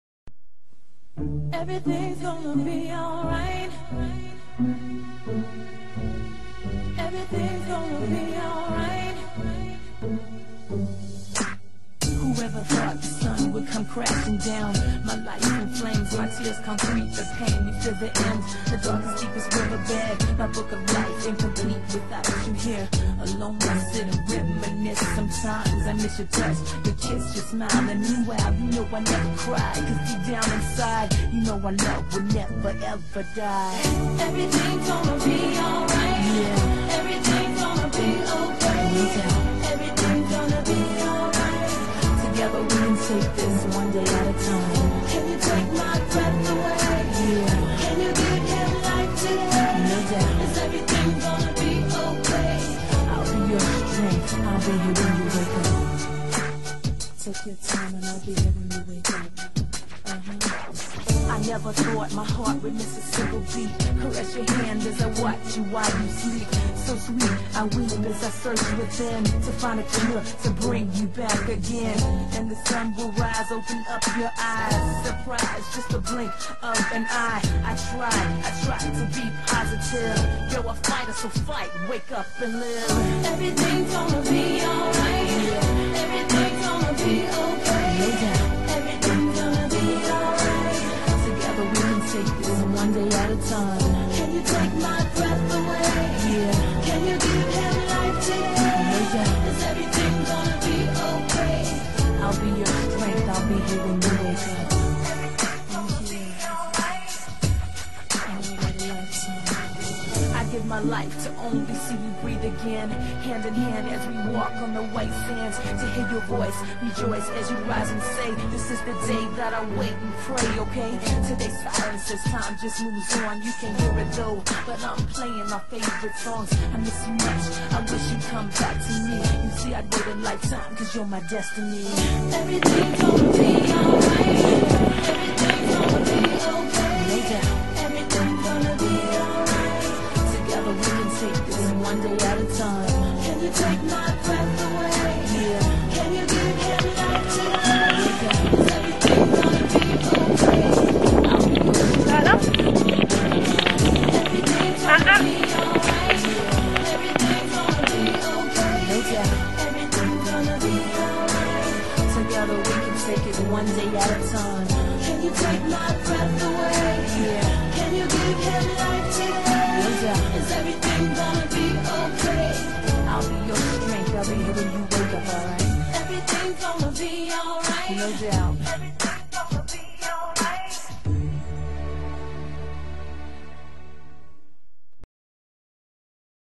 vill.wmv
first days in El Paso and having a saddle on his back